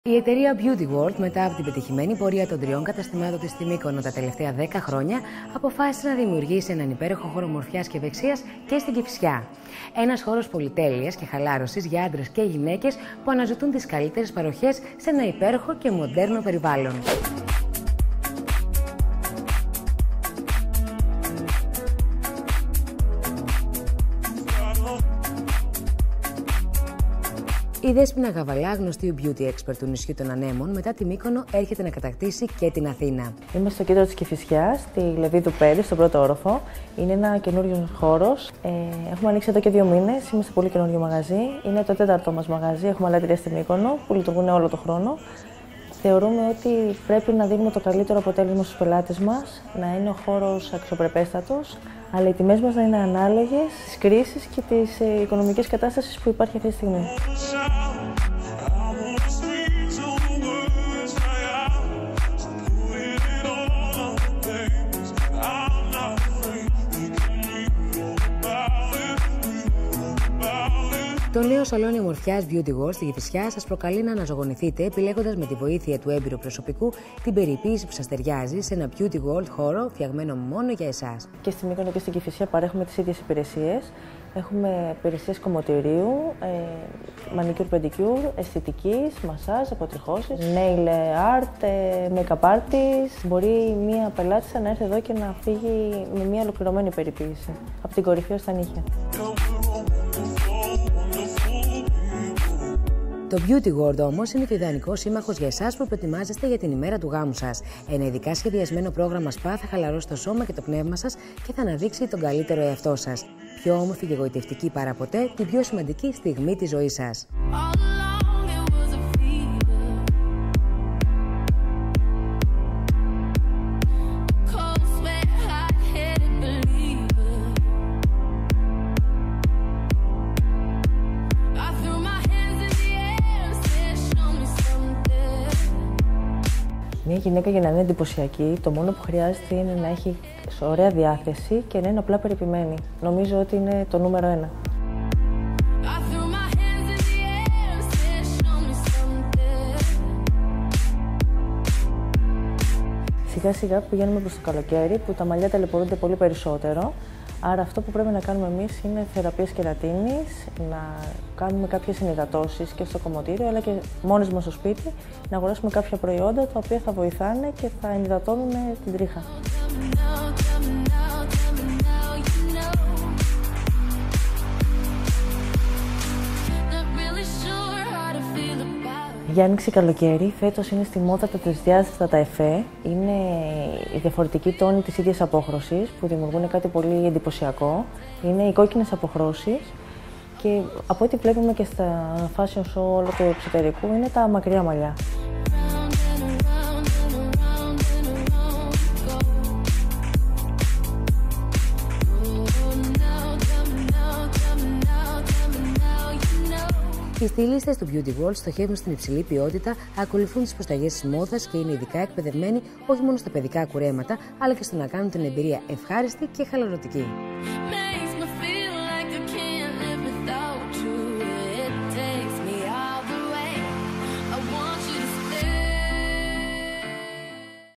Find4me.gr: Beautyworld
Beautyworld by Despina Gavala Nails & Hair Salon
Addresses:
Drafaki, 84600, Mykonos

Athens: Kifisia, 5 Levidou St.

BeautyWorld
Η εταιρεία Beauty World μετά την πετυχημένη πορεία των 3 καταστημάτων της στην Μύκονο τα τελευταία 10 χρόνια , αποφάσισε να  δημιουργήσει έναν υπέροχο χώρο ομορφιάς και ευεξίας και στην Κηφισιά. Ένας χώρος πολυτελείας και χαλάρωσης για άντρες και γυναίκες που αναζητούν  τις καλύτερες παροχές σε ένα υπέροχο και μοντέρνο περιβάλλον.